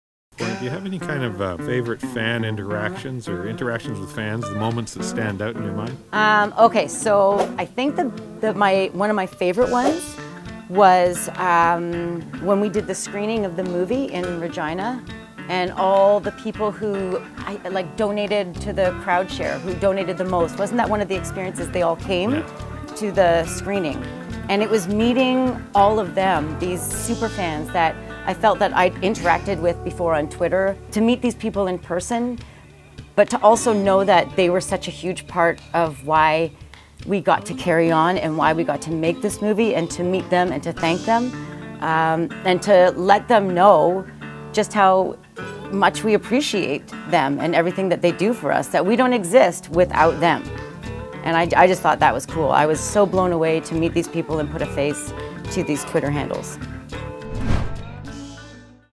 Tara Spencer Nairn Recounts Her Favourite Fan Moment in Regina | Corner Gas Cast Interview
Tara Spencer-Nairn recounts that her favourite fan moment took place at a special screening of Corner Gas: The Movie in Regina, Saskatchewan. "We don't exist without [the fans] ... I was so blown away to meet them..." she says.

WHERE TO WATCH CORNER GAS + CORNER GAS ANIMATED

AMAZON PRIME VIDEO

IN AUSTRALIA, NEW ZEALAND + UK:
AMAZON PRIME VIDEO